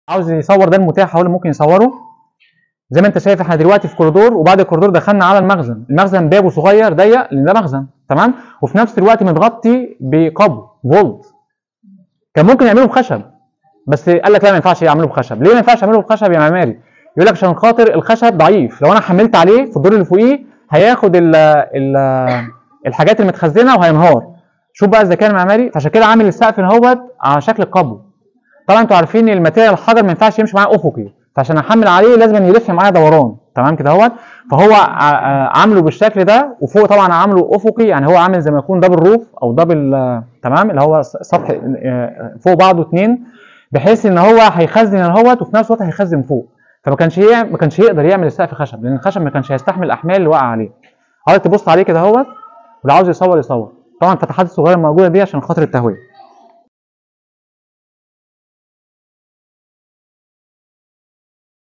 شرح نموذج مخرن بوكالة الغورى بالغورية وعرض فكرة التحميل من خلال السقف على شكل قبو, د/كمال الجبلاوى
السلام عليكم ورحمة الله وبركاته, ويارب الجميع يكون دائما بخير,
شرح نموذج مخرن بوكالة الغورى بالغورية وعرض فكرة التحميل من خلال السقف على شكل قبو,
أتمنى من الله عز وجل أن يكون علم ينتفع به إلى يوم القيامة,
خالص الشكر والتقدير لكل المجتهدين, د/كمال محمود الجبلاوى
History of Islamic Architecture,
Dr/ Kamal Elgabalawy